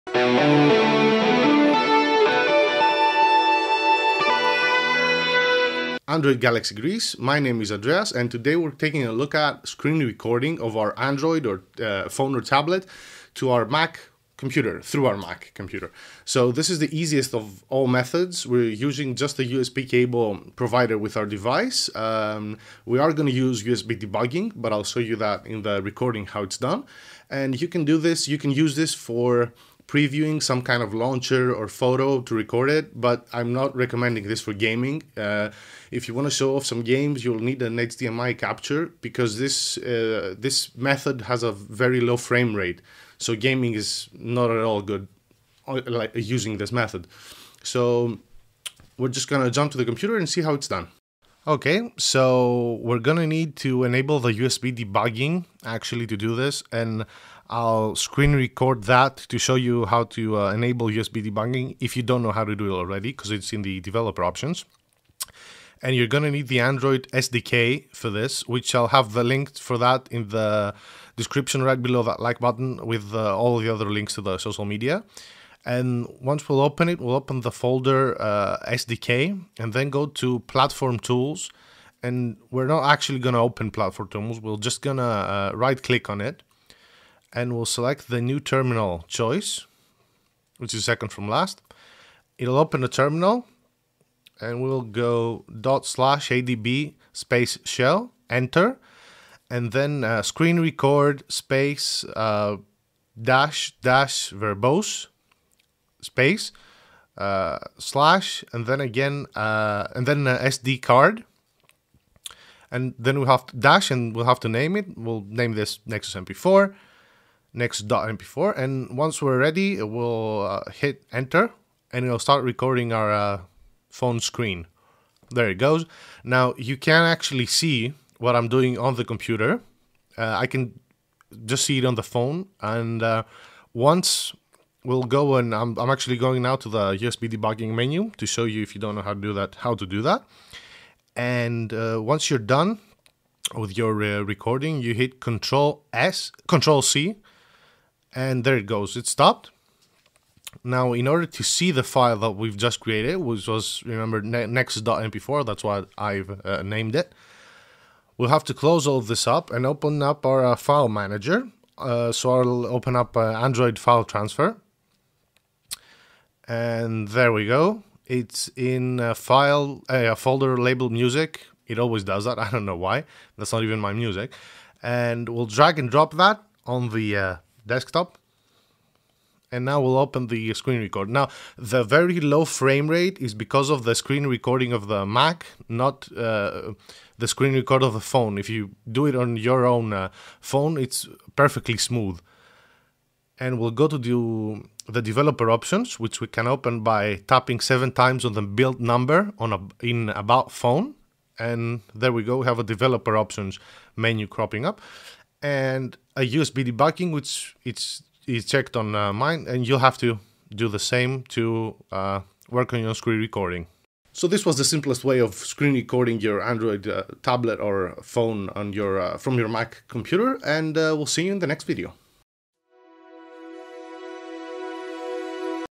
How to record your Android screen on a Mac - Android Galaxy Greece
The terminal commands are:
./adb shell
screenrecord --verbose /sdcard/nameyourfile.mp4
Press Control & C once you're finished